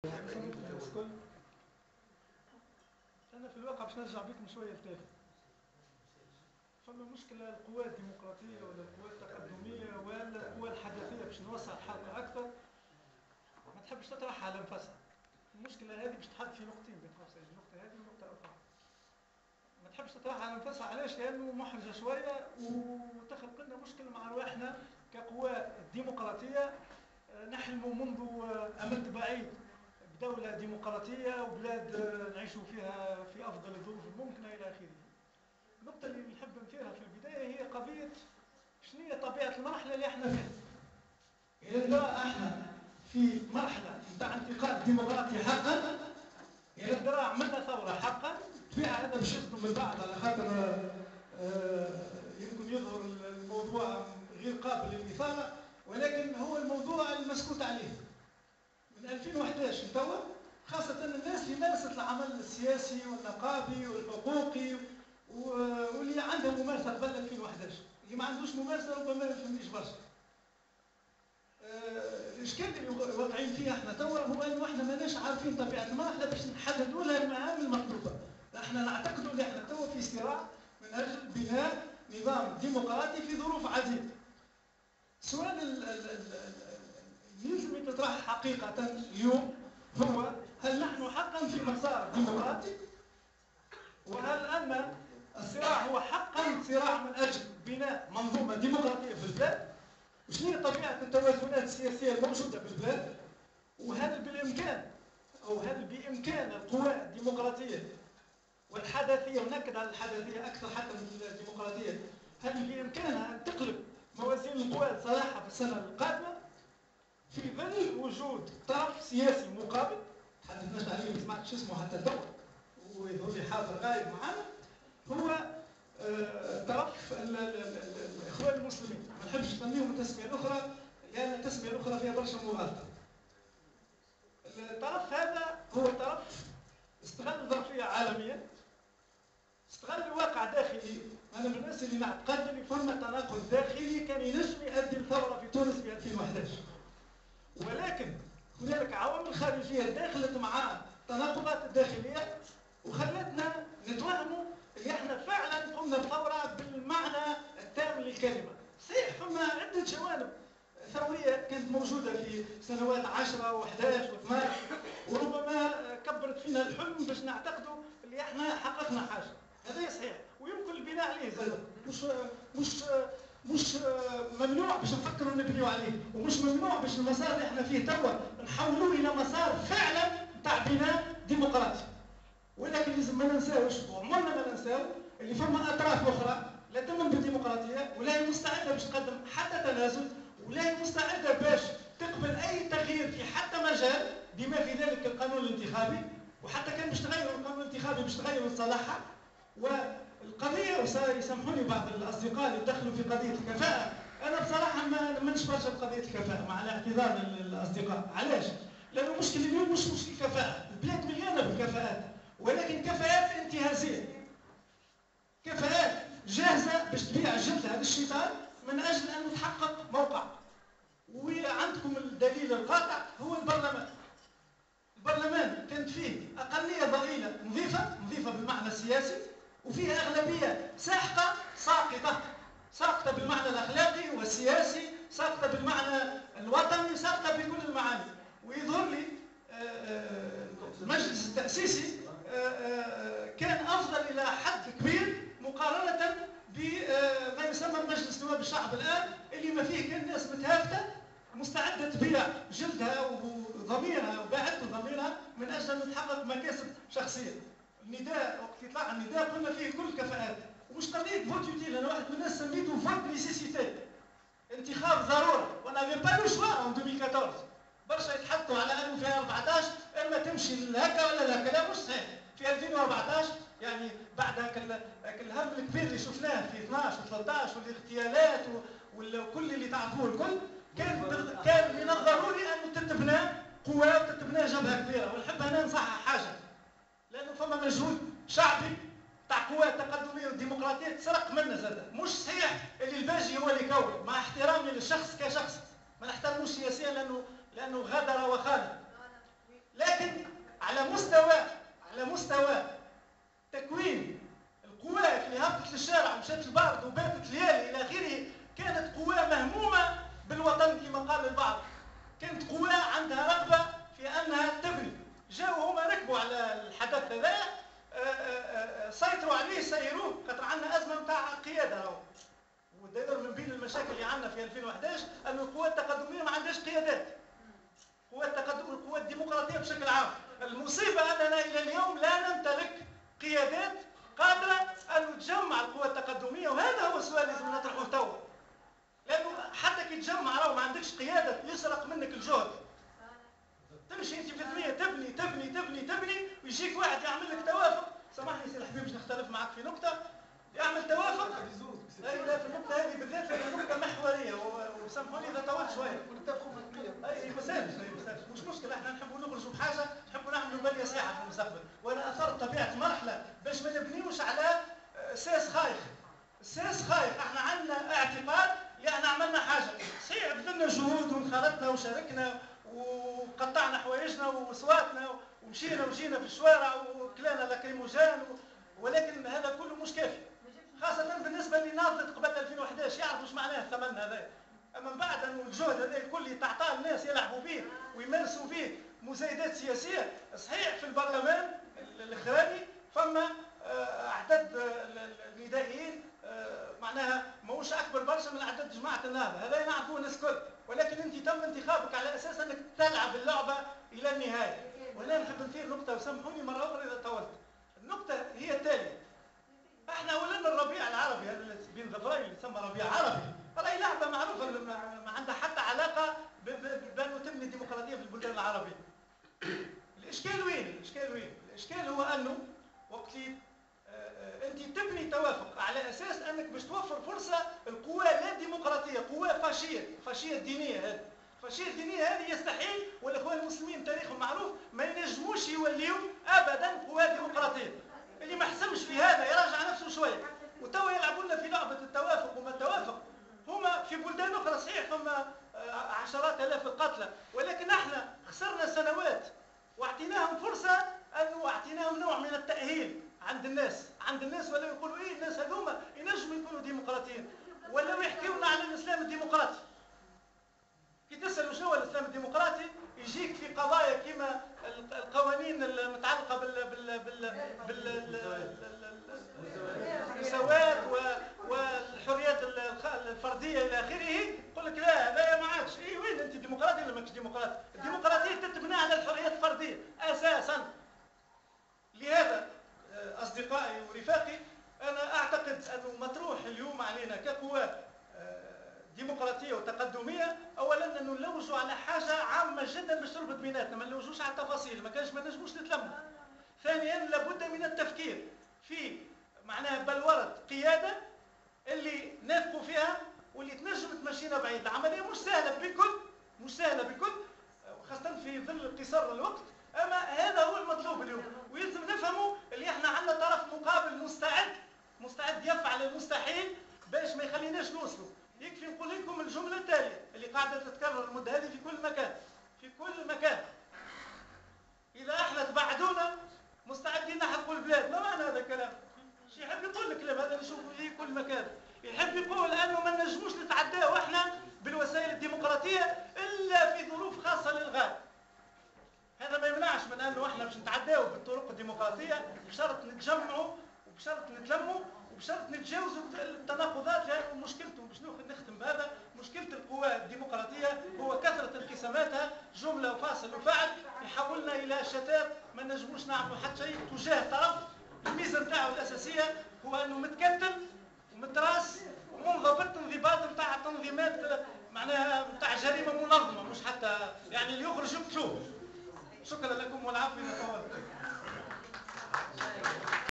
CAFE POLITIQUE  du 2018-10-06 à Menzel Bourguiba avec la participation des militants de Bizerte, Sejnane, Ras Djebel, Mateur, Ain Drahem